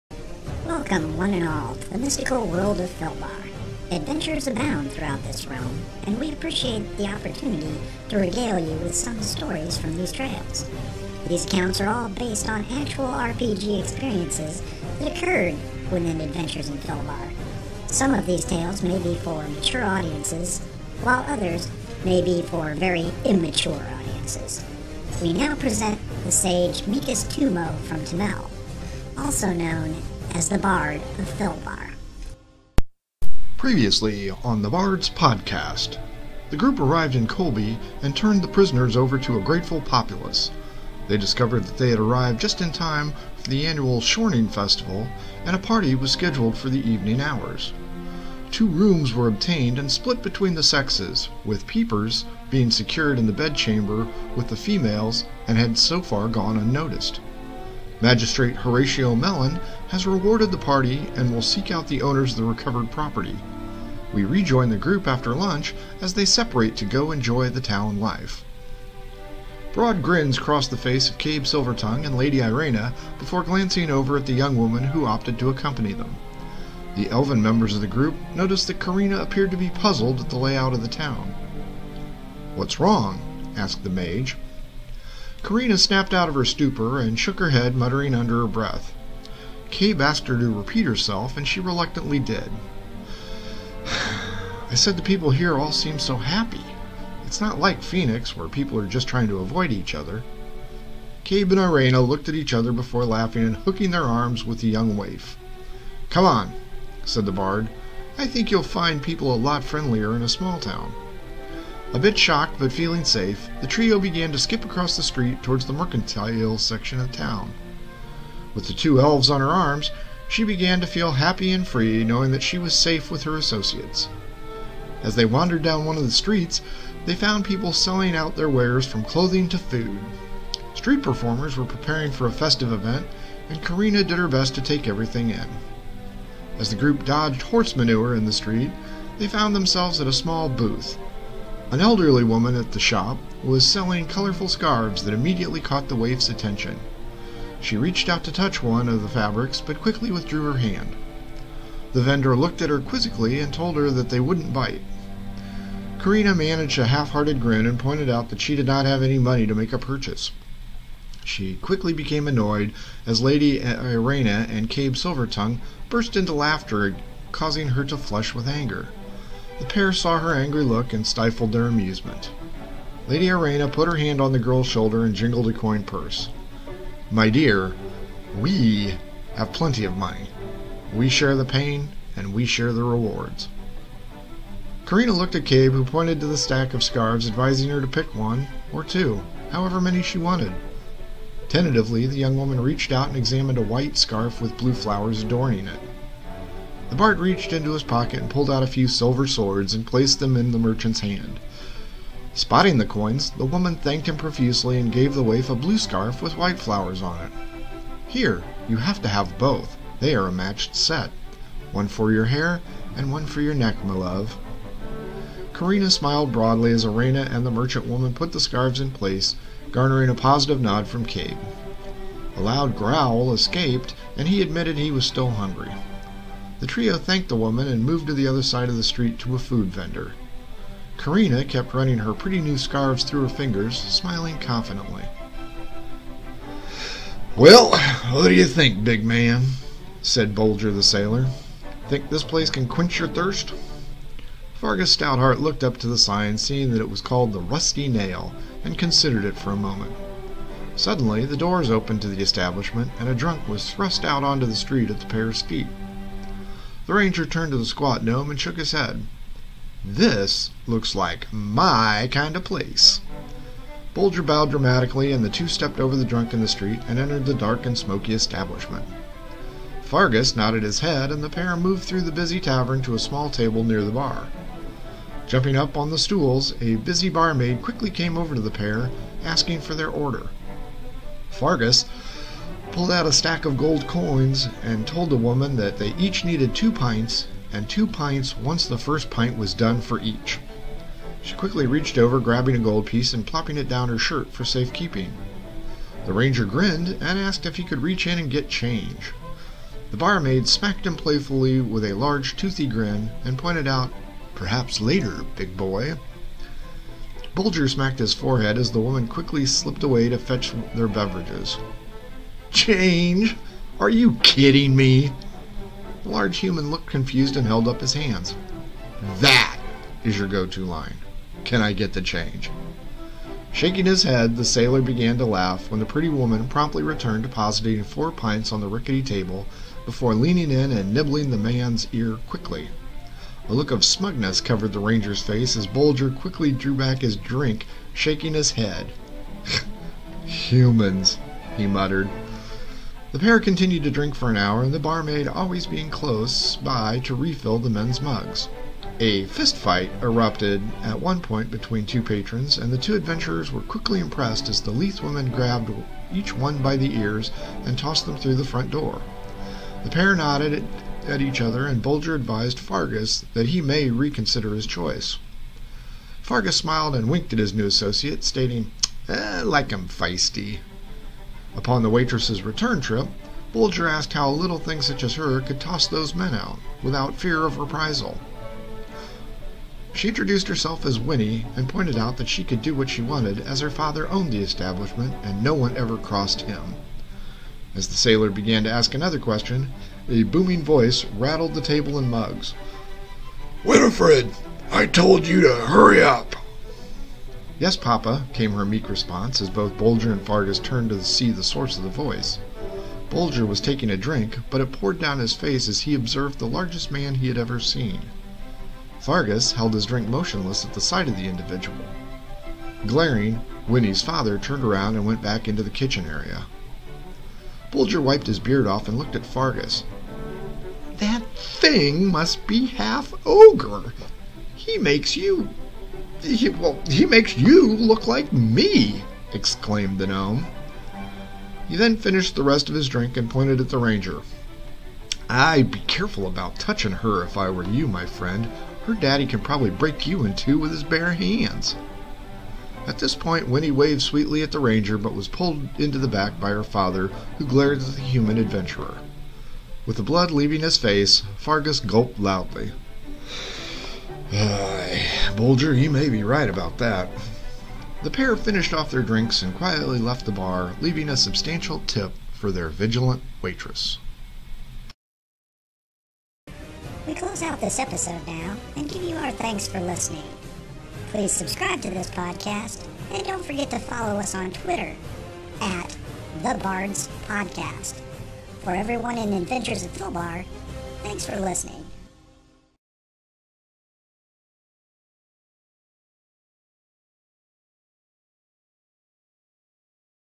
Session Fartook - 44 Party Time
The group settles in to their rooms and opt to explore the town in different directions. Karina is surprised by her financial status.